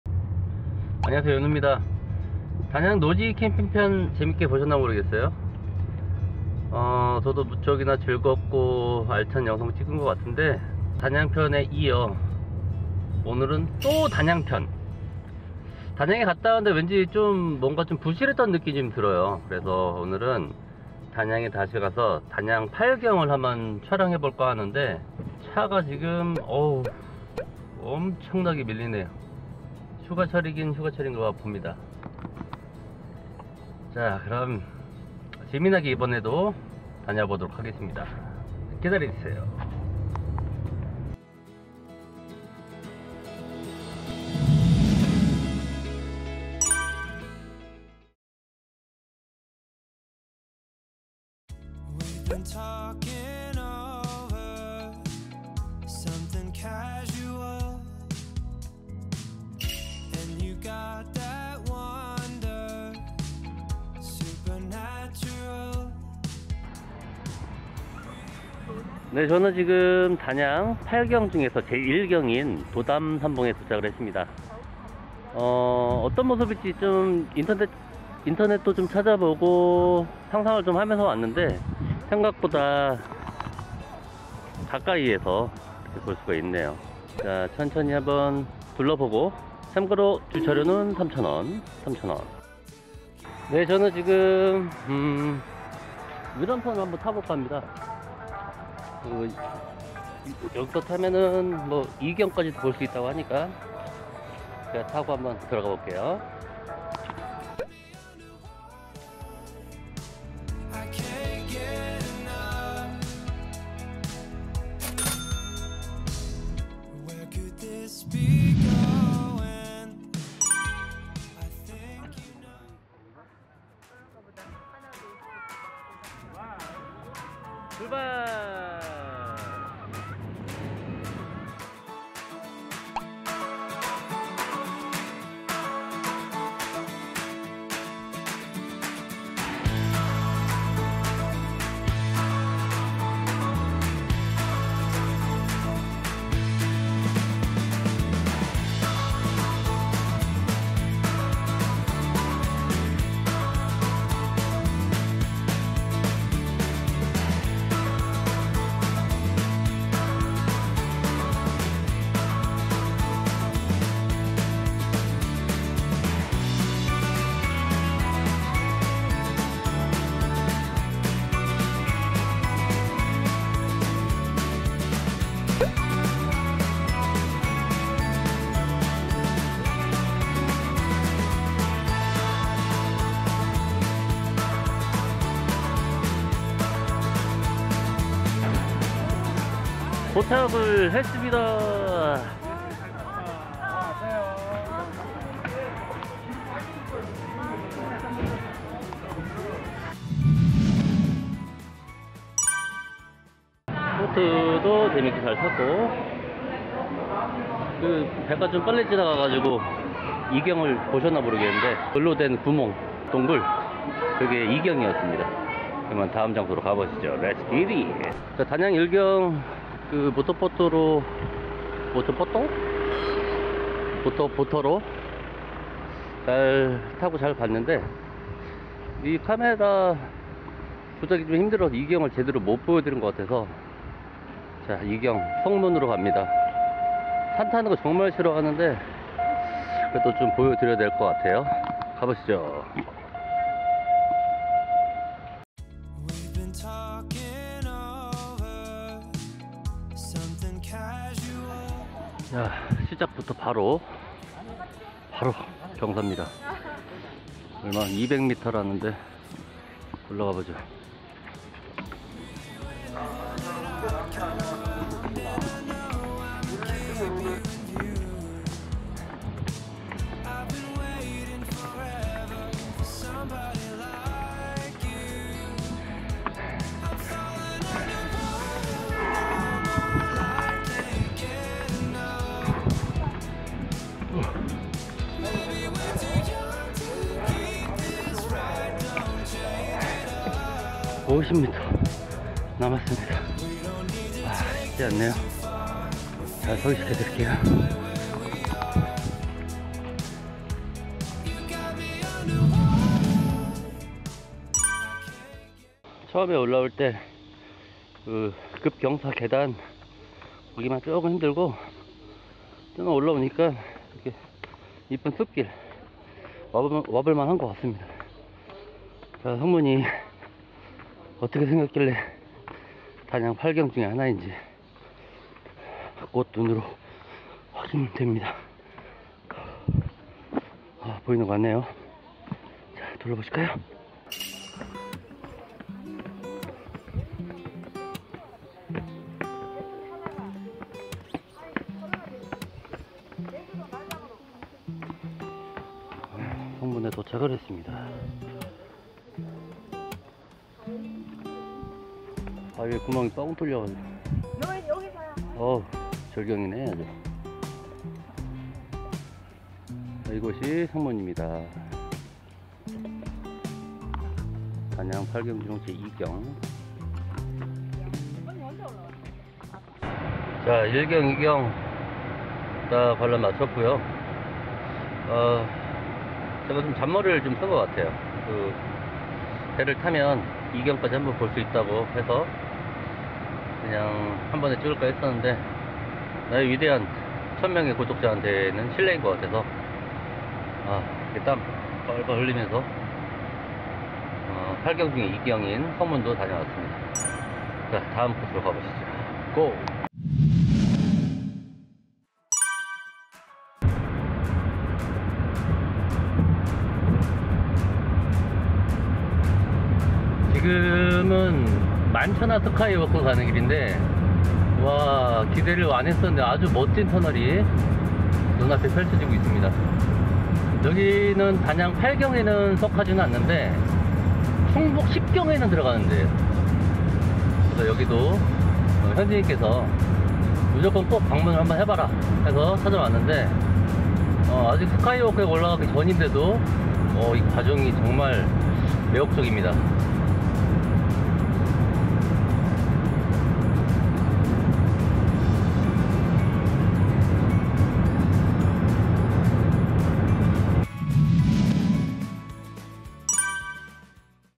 단양여행 필수코스 단양팔경 시작합니다~도담삼봉과 석문의 만남#연우라이프
컴퓨터도 문제가 있었고 명절이 겹쳐 업로드가 살짝 늦었습니다
또 단양이라 실망 하신 건 아니죠?
너무 놀고 먹기만 한 거 같아 다시금 단양을 찾아
진정 가볼만 한 여행지를 정리해 봤습니다
단양팔경의 순서는 개의치 마세요^^ 그래야 다 팔경 안에 있습니다~
연우라이프와 떠나보는 단양의 구석구석 쭉쭉 이어집니다